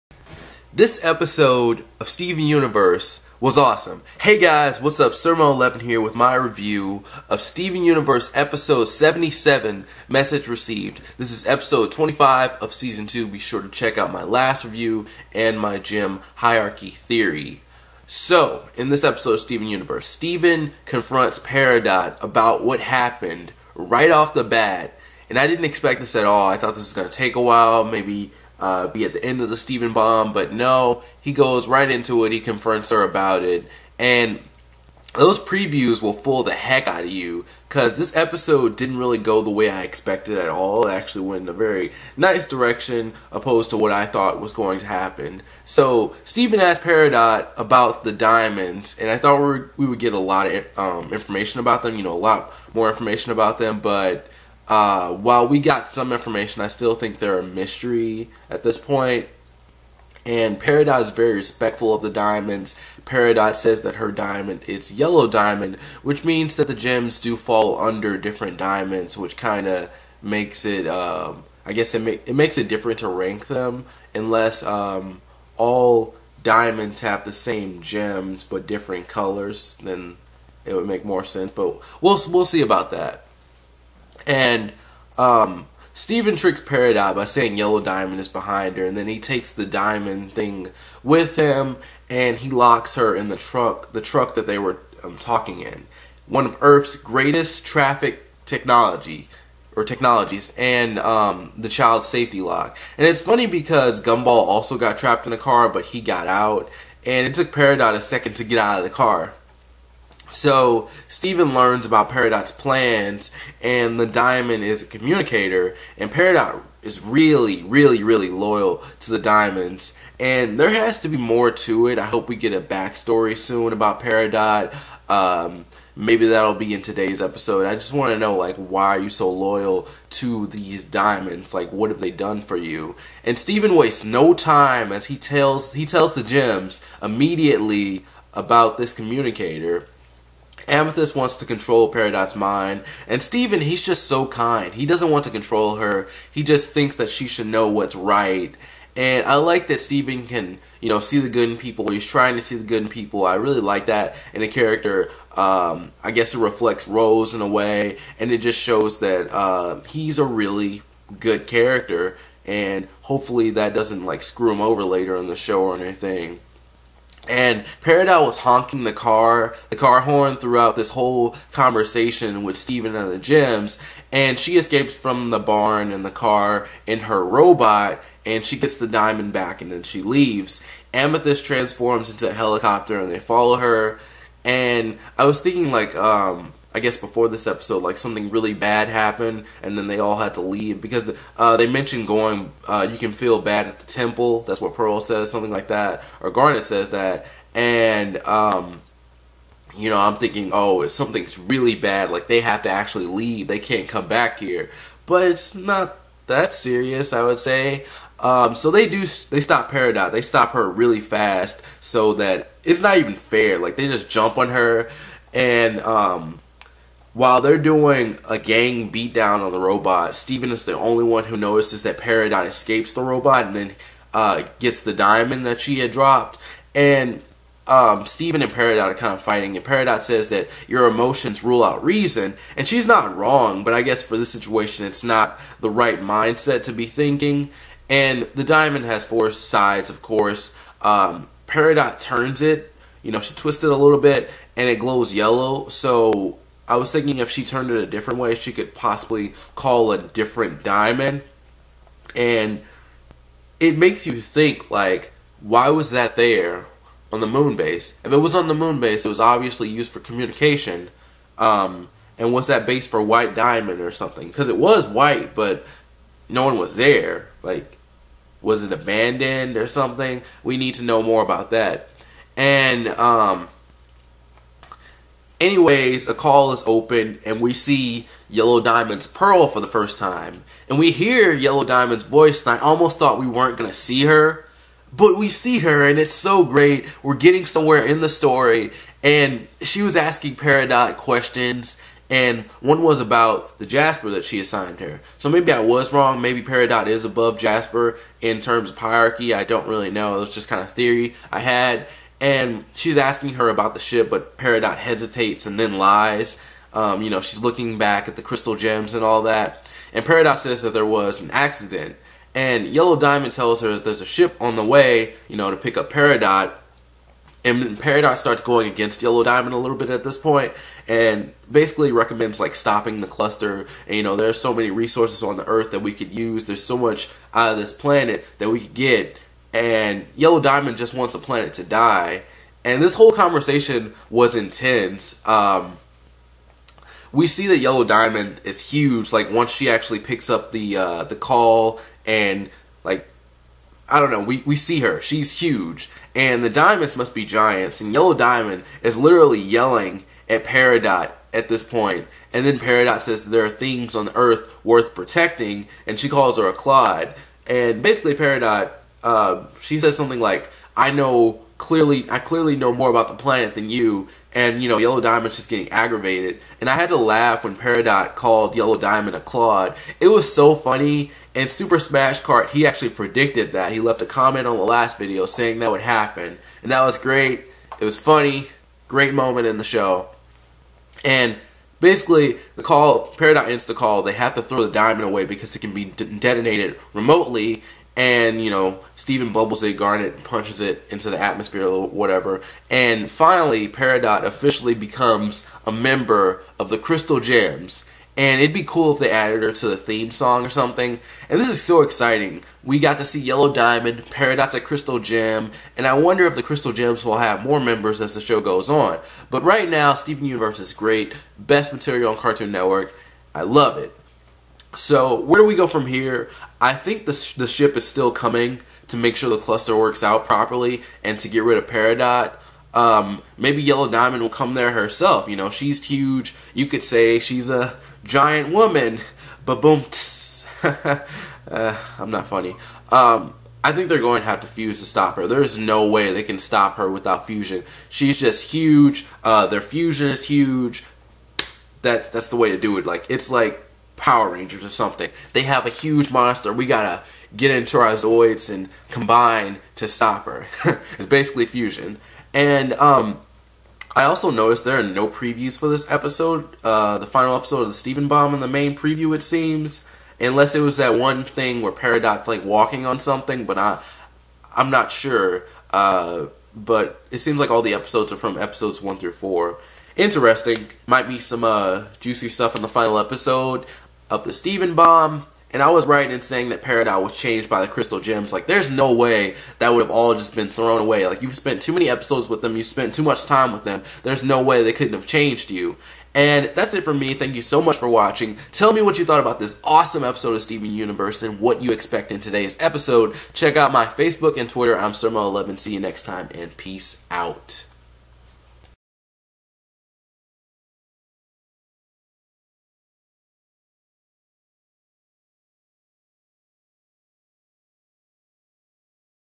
Steven Universe "Message Received" Episode 77 Review YELLOW DIAMOND REVEALED
Sirmel11 reviews Steven Universe Season 2 Episode 28 aka Steven Universe Episode 77 "Message Received".

We finally get to see what Yellow Diamond looks like. Are you hyped? Crystal Gems vs Yellow Diamond is coming soon?

Where you can find Sirmel11